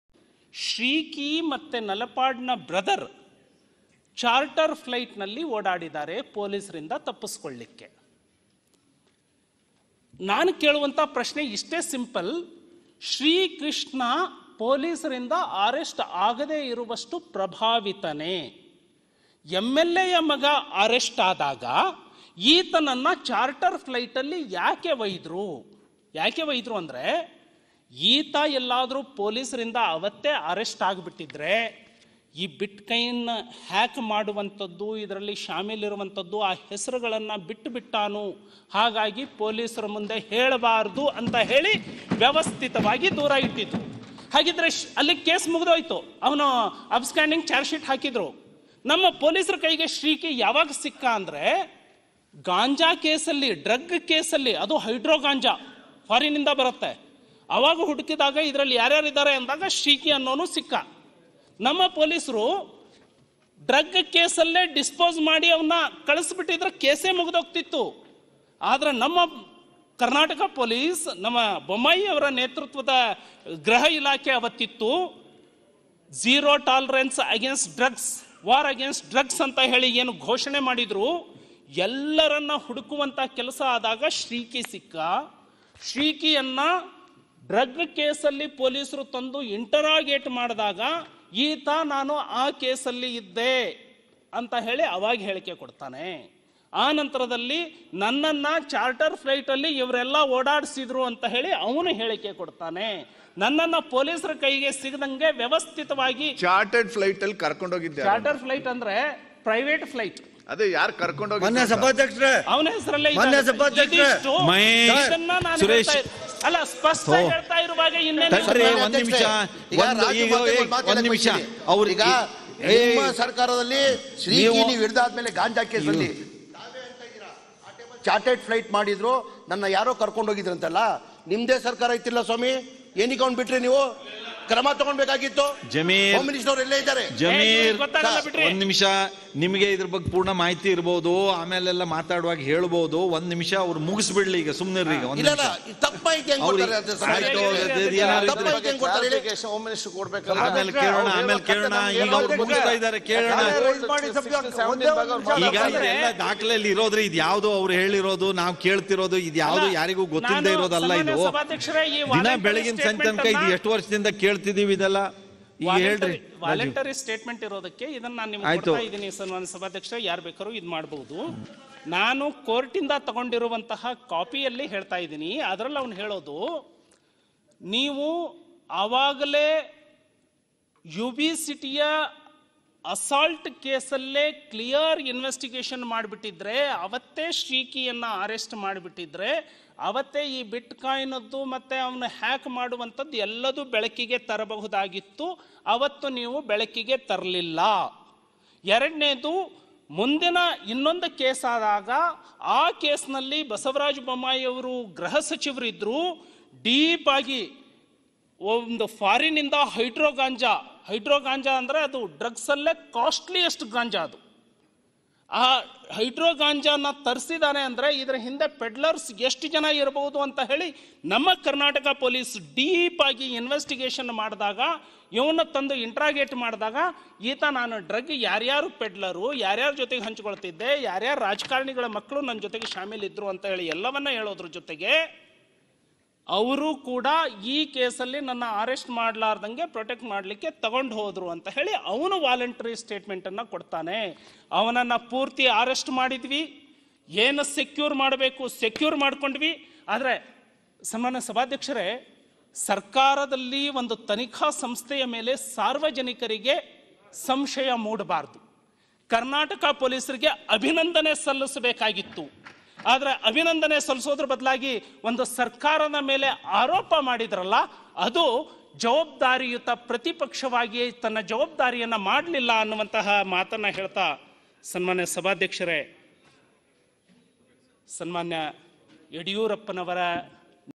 Karnataka Assembly 2022 : P Rajeev Vs Zameer Ahmed Khan Talk War in Assembly 2022 | Modala Suddi TV
Latest breaking news and exclusive interviews from Karnataka, only on Modala Suddi TV